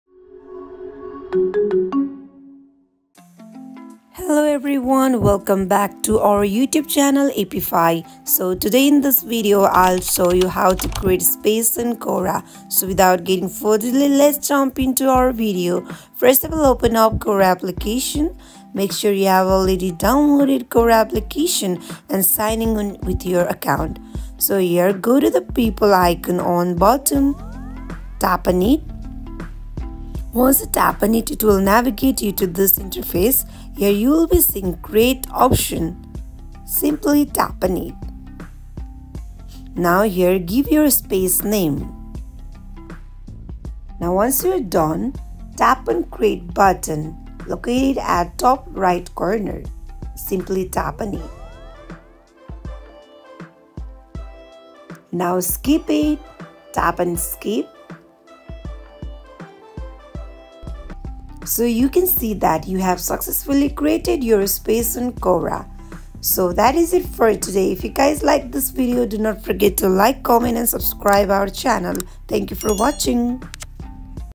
How to Create Spaces on Quora (2024) | Quora Space Tutorial
Master the art of creating engaging Quora Spaces in 2024 with this expert tutorial. Discover how to build and manage thriving online communities around your niche.